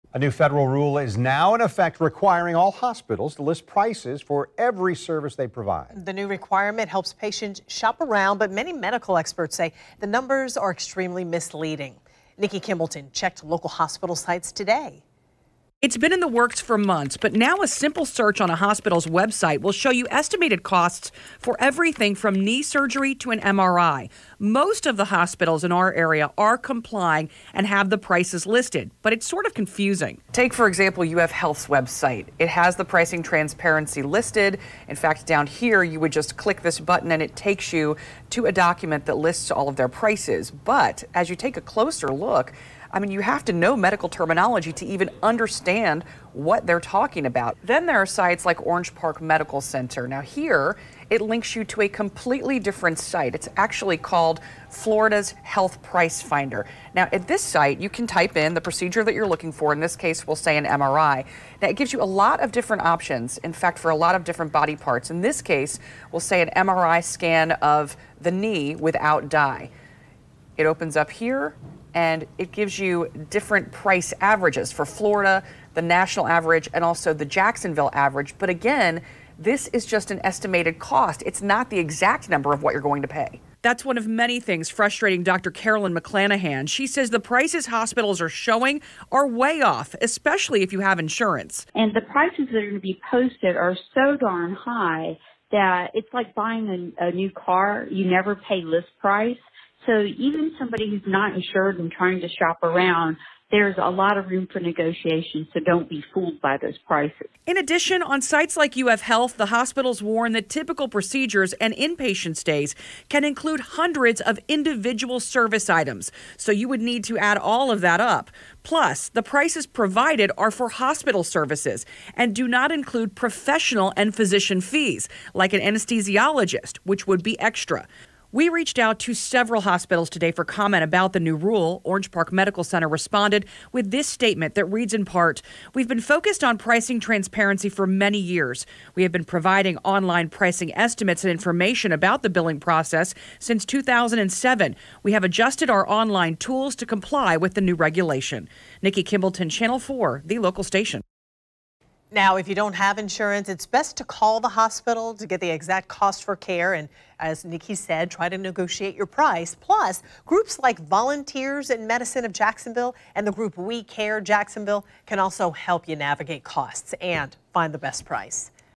Misleading hospital prices according to experts
Many medical experts state the prices of hospital services are extremely misleading.